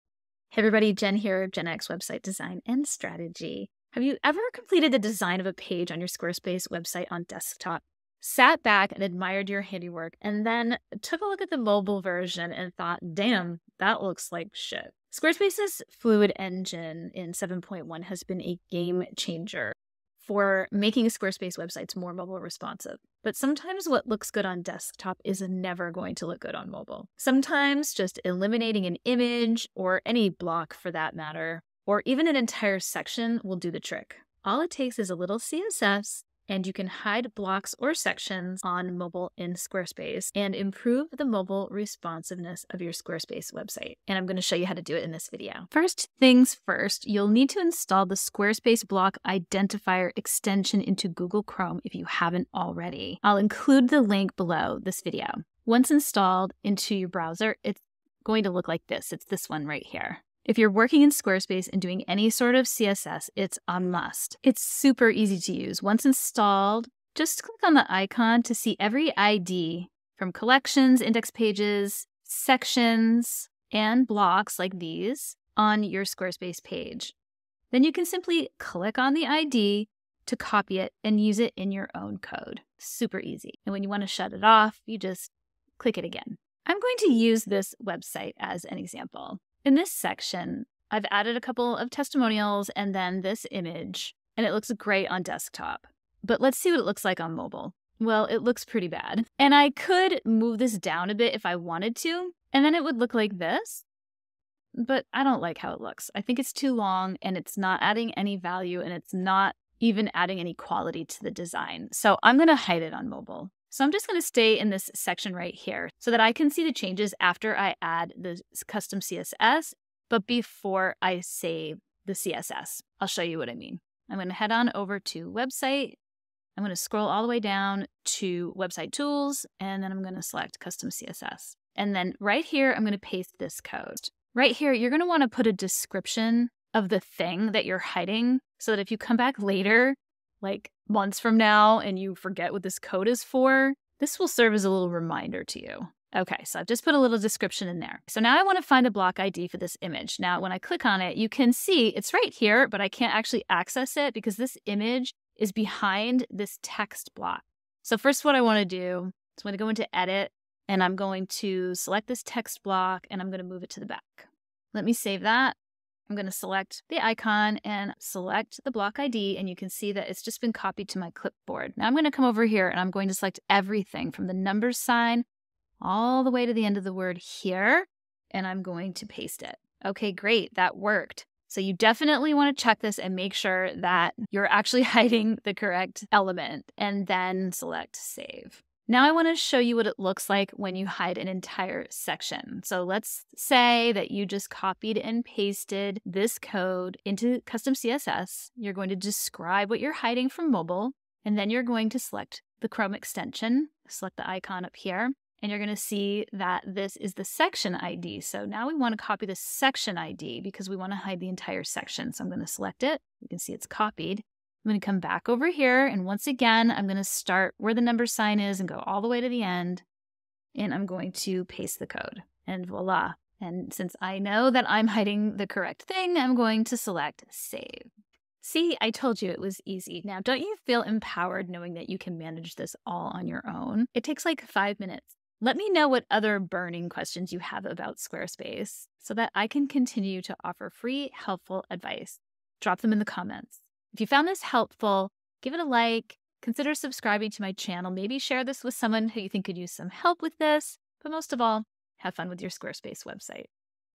How to Hide Blocks and Sections On Mobile in Squarespace
Squarespace’s Fluid Engine in 7.1 has been a game-changer for making Squarespace website’s more mobile responsive, but sometimes what looks good on desktop will never look good on mobile.

All it takes is a little CSS and you can hide blocks and sections on mobile in Squarespace and improve the mobile responsiveness of your Squarespace website.

🆓🎓 FREE CLASS: The 3-Part Framework for Increasing Organic Traffic to Your Squarespace

CODE SNIPPET:

/* Describe what you're hiding from
mobile so you don't forget*/

@media screen and (max-width: 768px) {
block or section id goes here {
display:none !important;

0:45 First things first, install the Squarespace Block extension on your browser
1:37 View your design on mobile view
2:05 Adding custom CSS
3:16 Hide sections

👉 The Best Way to Share YouTube Videos in Your Squarespace Blog Posts